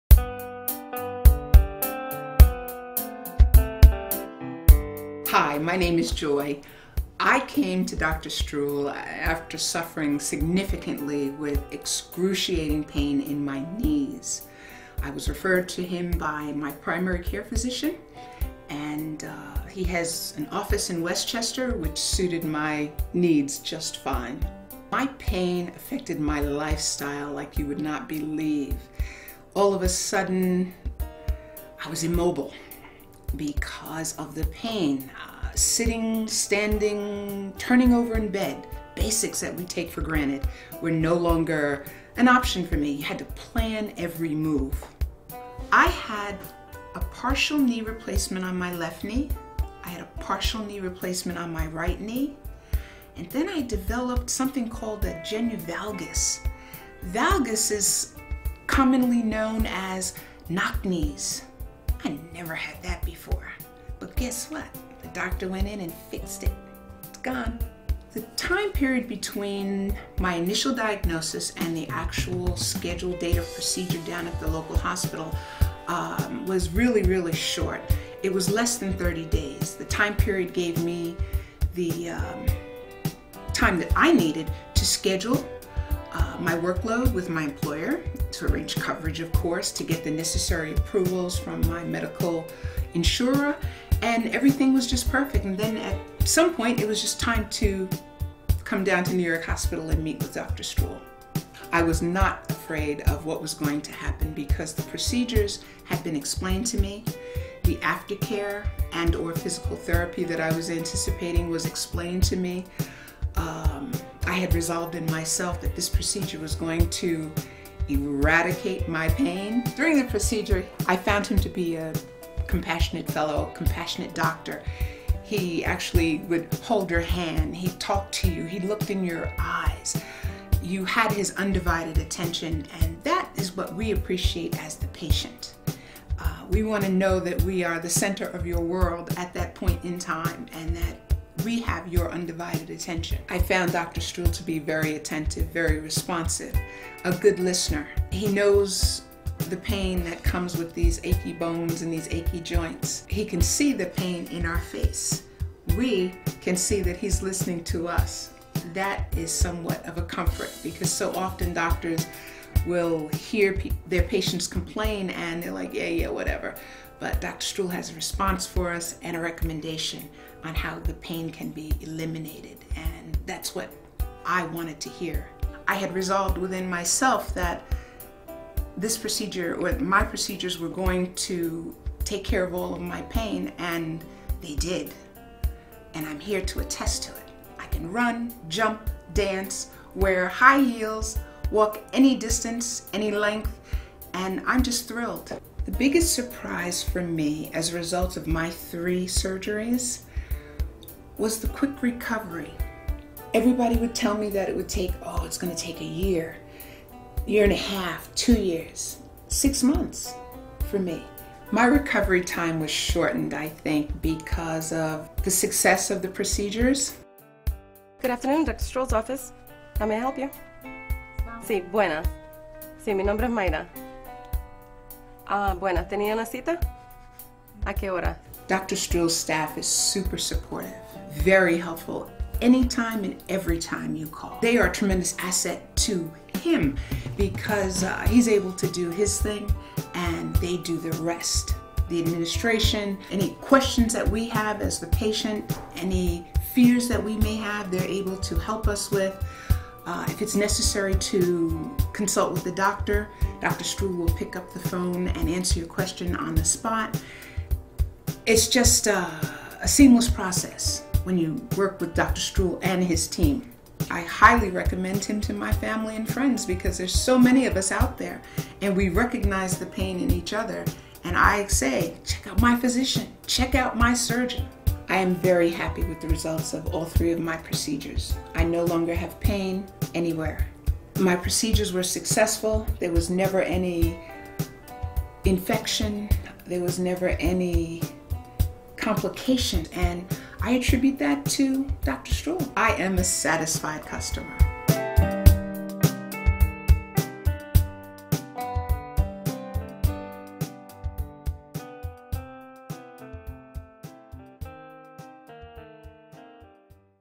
Partial Knee Replacement Testimonial by Joy - Dr. Steven Struhl
Partial knee replacement patient Joy talks about her Genu Valgum knee experience and after surgery lifestyle. Joy talks about how Dr. Steven Struhl helped her to eliminate her valgus knee pain, to have quicker recovery period, and to retain her mobility.